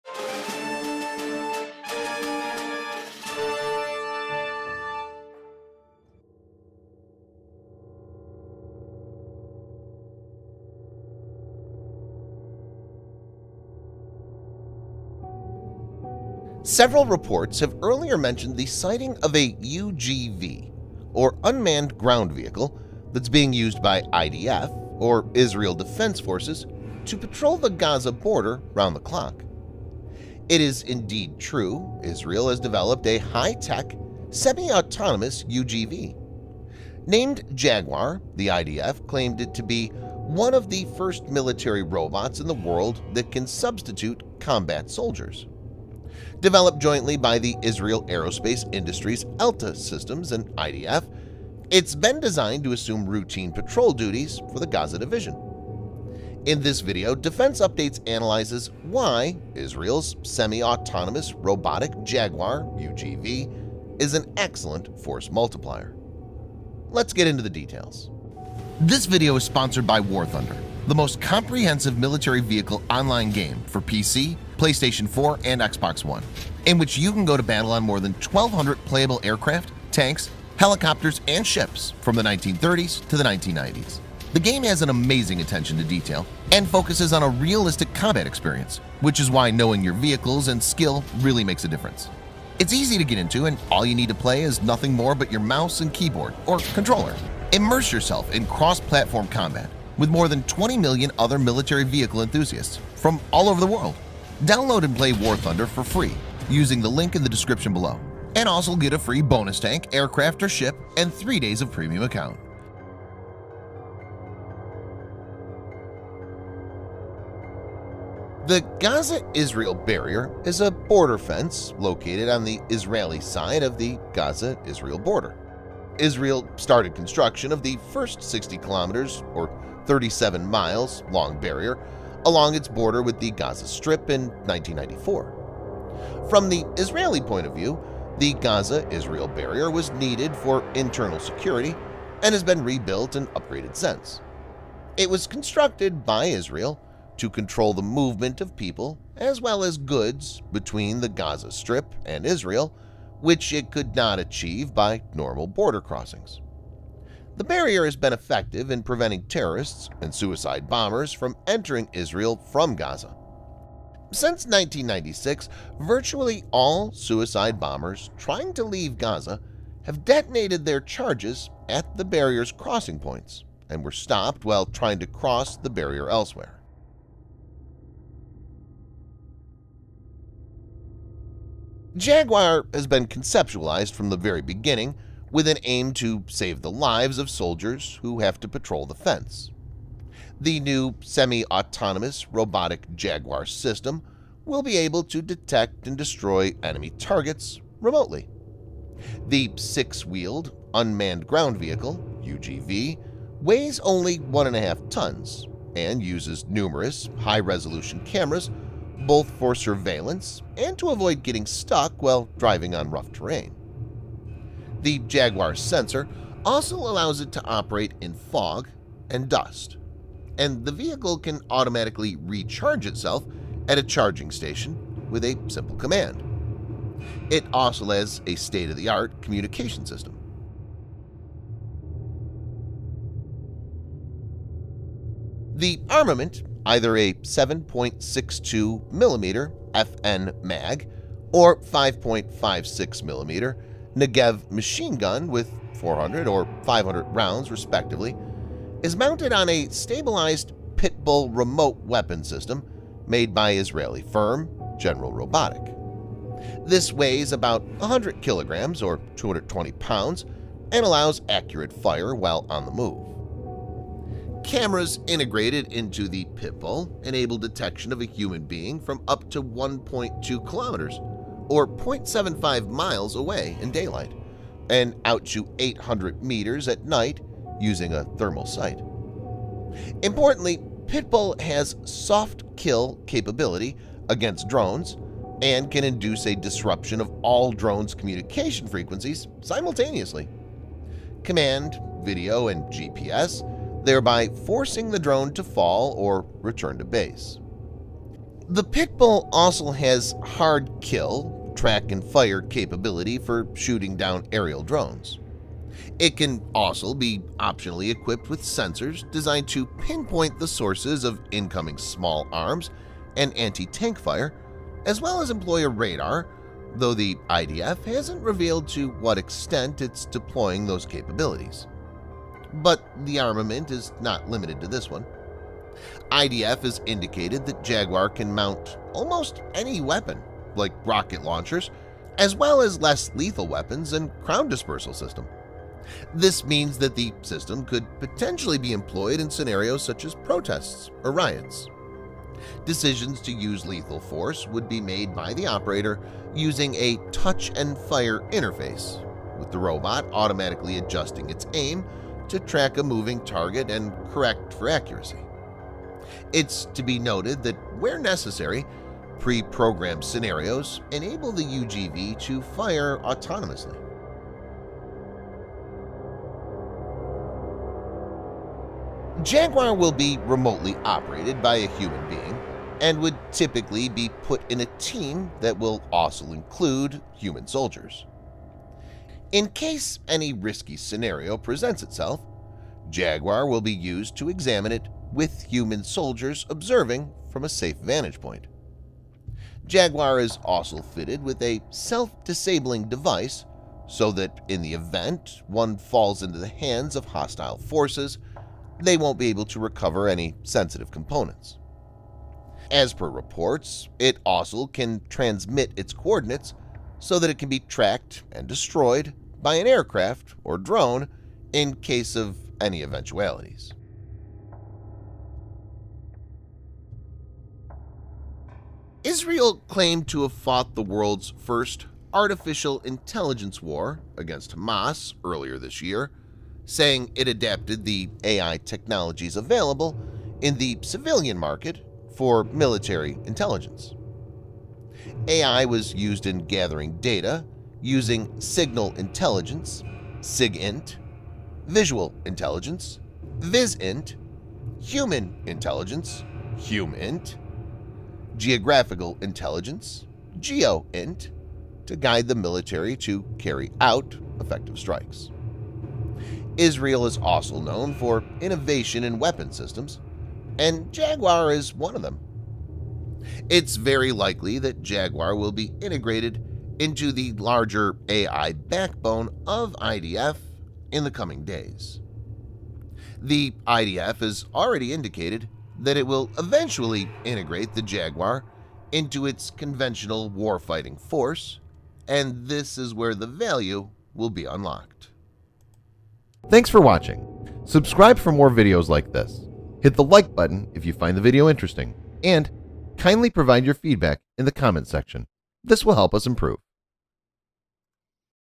Israel's Jaguar Unmanned Ground Vehicle is patrolling Gaza border !
Several reports have earlier mentioned the sighting of a UGV or Unmanned Ground Vehicle that is being used by IDF or Israel Defense Forces to patrol the Gaza border round the clock.

It is indeed true, Israel has developed a high-tech semi-autonomous UGV
Named ‘Jaguar’, the IDF claims it to be “one of the first military robots in the world that can substitute combat soldiers”.

Developed jointly by the Israel Aerospace Industries’ Elta systems and IDF, it has been designed to assume routine patrol duties for the Gaza division.

In this video Defense Updates analyzes why Israel's semi-autonomous robotic ‘Jaguar’ UGV is an excellent force multiplier?

BACKGROUND MUSIC

IMAGES USES